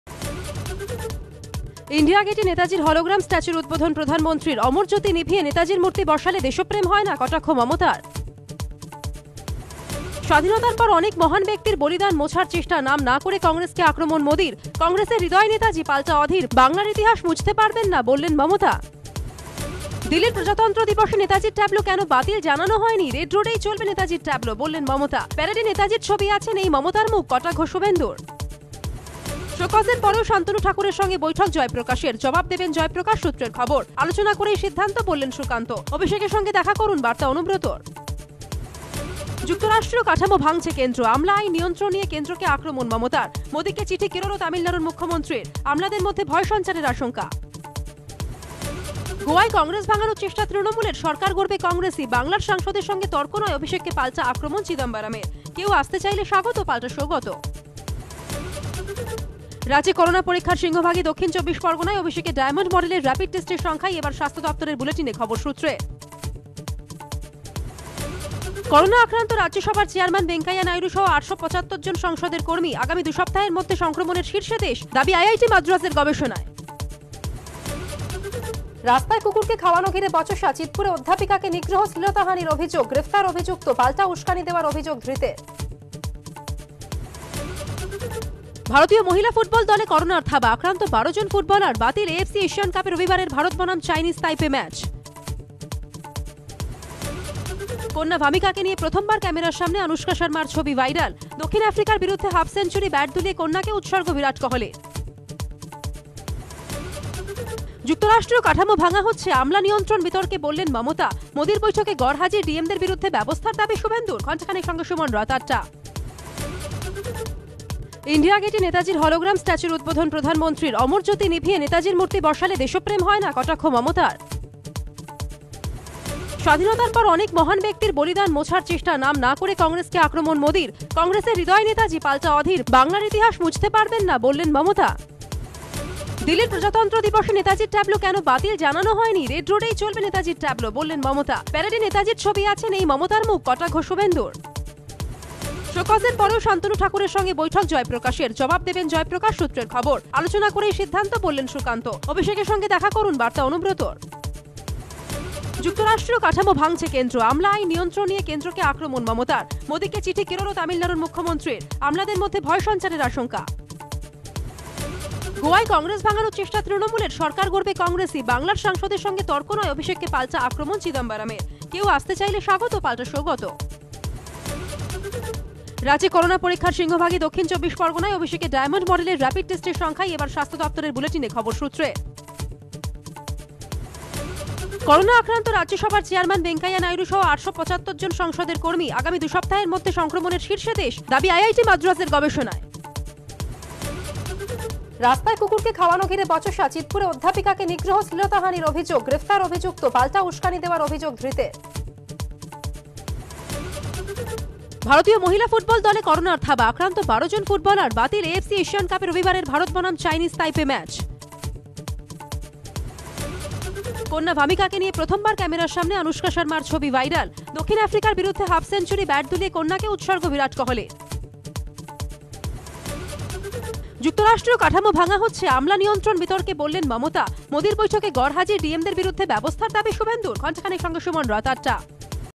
Morning Headlines: ইন্ডিয়া গেটে নেতাজির হলোগ্রাম স্ট্যাচুর উদ্বোধন প্রধানমন্ত্রীর। Bangla News
ইন্ডিয়া গেটে নেতাজির হলোগ্রাম স্ট্যাচুর উদ্বোধন প্রধানমন্ত্রীর (Narendra Modi)। অমর জ্যোতি নিভিয়ে নেতাজির মূর্তি বসালে দেশপ্রেম হয় না, কটাক্ষ মমতার (Mamata Banerjee)।

স্বাধীনতার পর অনেক মহান ব্যক্তির বলিদান মোছার চেষ্টা। নাম না করে কংগ্রেসকে (Congress) আক্রমণ মোদির। কংগ্রেসের হৃদয়ে নেতাজি, পাল্টা অধীর। বাংলার ইতিহাস মুছতে পারবেন না, বললেন মমতা (Mamata Banerjee)।

দিল্লির প্রজাতন্ত্র দিবসে নেতাজির ট্যাবলো কেন বাতিল জানানো হয়নি। রেড রোডেই চলবে নেতাজির ট্যাবলো, বললেন মমতা। প্যারেডে নেতাজির ছবি আছে, নেই মমতার মুখ। কটাক্ষ শুভেন্দুর (Suvendu Adhikari)।

শোকজের পরেও শান্তনু ঠাকুরের (Shantanu Thakur) সঙ্গে বৈঠক জয়প্রকাশের। জবাব দেবেন জয়প্রকাশ, সূত্রের খবর। আলোচনা করেই সিদ্ধান্ত, বললেন সুকান্ত। অভিষেকের (Abhishek Banerjee) সঙ্গে দেখা করুন, বার্তা অনুব্রতর (Anubrata Mondal)।

যুক্তরাষ্ট্রীয় কাঠামো ভাঙছে কেন্দ্র। আমলা-নিয়ন্ত্রণ আইন নিয়ে কেন্দ্রকে আক্রমণ মমতার। মোদিকে চিঠি কেরল ও তামিলনাড়ুর মুখ্যমন্ত্রীর। আমালাদের মধ্যে ভয় সঞ্চারের আশঙ্কা।

গোয়ায় কংগ্রেস ভাঙানোর চেষ্টা তৃণমূলের, ম্যাটার ক্লোজড। জোট প্রস্তাবে কার্যত দাঁড়ি টেনে অভিষেককে জবাব চিদম্বরমের। তৃণমূল কাউকে ভাঙায় না, অনেকে কংগ্রেস ছেড়েছেন নিজেই, পাল্টা সৌগত।

রাজ্যে করোনায় দৈনিক সংক্রমণ কমে প্রায় সাত হাজার। ফের তিরিশ পার করল মৃত্যুর সংখ্যা। তৃতীয় ঢেউয়ে সংক্রমণের শীর্ষ পেরিয়েছে কলকাতা। দাবি আইআইটি মাদ্রাজের গবেষণায়।

ভারতীয় মহিলা ফুটবল দলে করোনার থাবা। আক্রান্ত ১২ জন ফুটবলার। বাতিল  এএফসি এশিয়ান কাপে রবিবারের ভারত বনাম চাইনিজ তাইপে ম্যাচ।

যুক্তরাষ্ট্রীয় কাঠামো ভাঙা হচ্ছে, আমলা নিয়ন্ত্রণ বিতর্কে বললেন মমতা। মোদির বৈঠকে গরহাজির ডিএম-দের বিরুদ্ধে ব্যবস্থার দাবি শুভেন্দুর। ঘণ্টাখানেক সঙ্গে সুমন, রাত ৮।